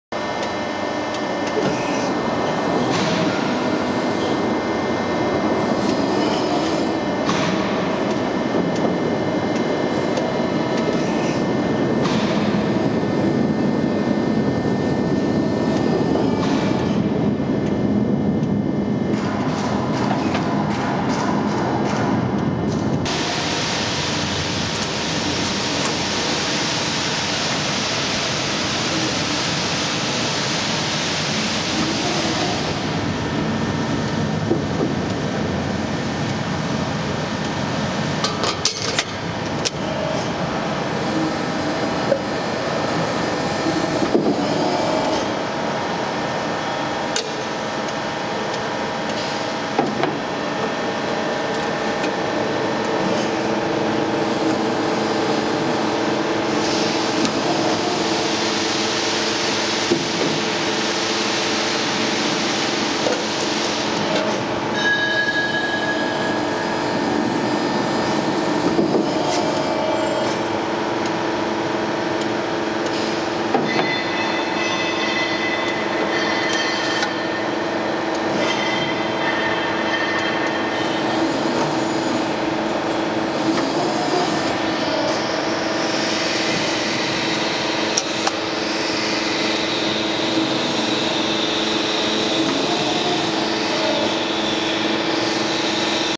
Hemming bench on press brake
Gasparini double height hemming bench with pneumatic operation.